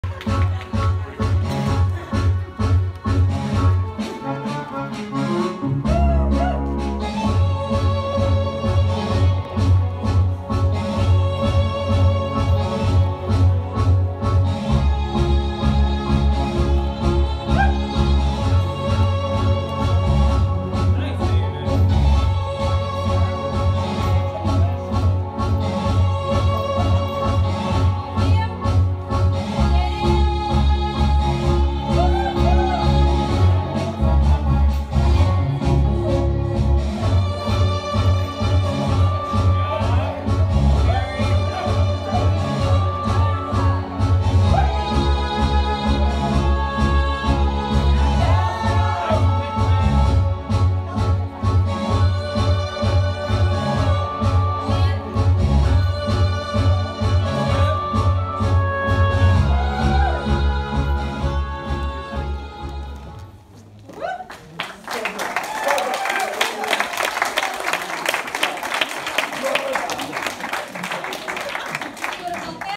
My Father and Anastasiia Dancing Tango Together
@FredAstaireDanceStudios Christmas Holiday Ball. Yep the instructors I got assigned were also assigned to my father.